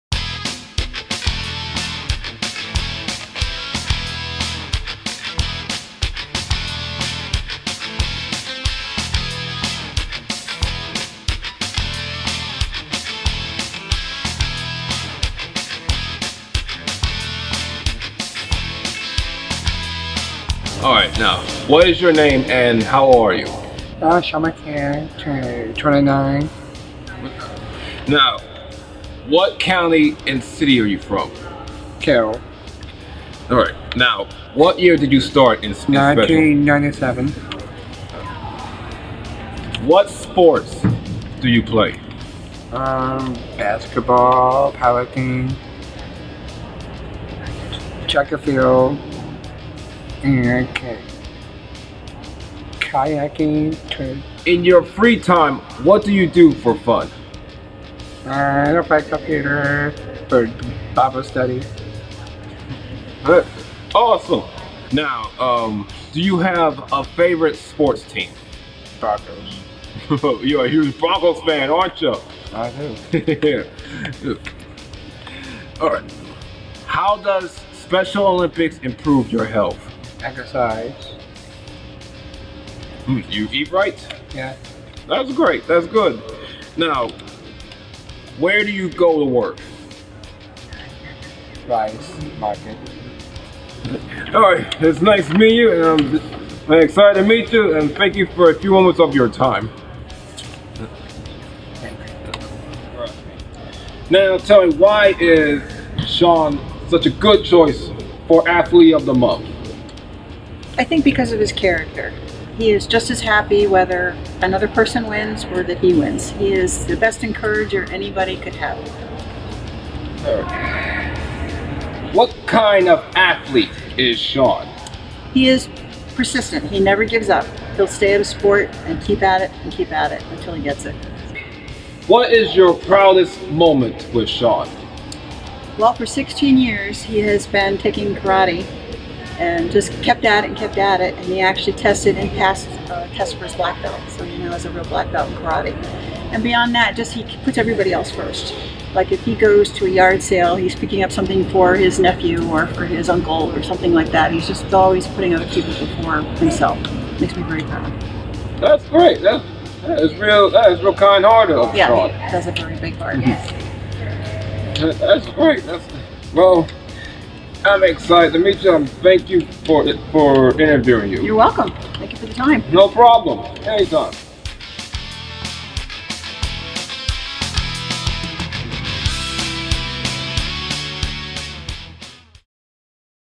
October SOMD Athlete of the Month - Sean McCann
Meet Sean McCann., Special Olympics Maryland's October Athletes of the Month!  Every month, we feature an athlete or athletes that has great leadership and helps further our mission. See what Sean is all about!